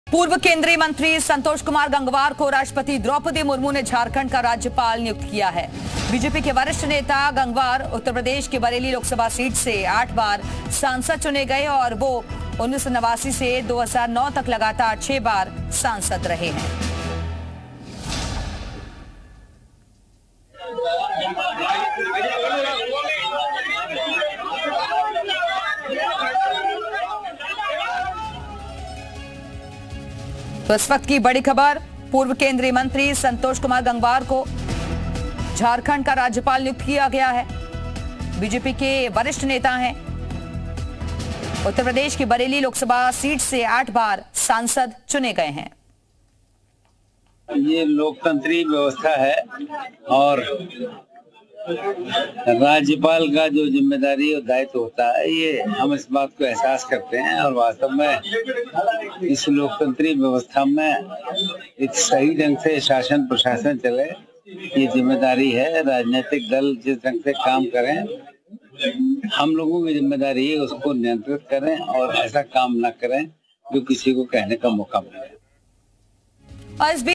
Governor of Jharkhand : झारखंड के राज्यपाल बने संतोष गंगवार | Santosh Gangwar | Breaking News
Governor of Jharkhand : झारखंड के राज्यपाल बने संतोष गंगवार, राष्ट्रपति द्रौपदी मुर्मू ने की नियुक्ति, बरेली से 8 बार सांसद रहे हैं संतोष गंगवार, श्रम एवं रोजगार मंत्री भी रहे हैं गंगवार

Governor of Jharkhand: Santosh Gangwar became the Governor of Jharkhand, appointed by President Draupadi Murmu, Santosh Gangwar has been MP from Bareilly 8 times, Gangwar has also been the Labor and Employment Minister.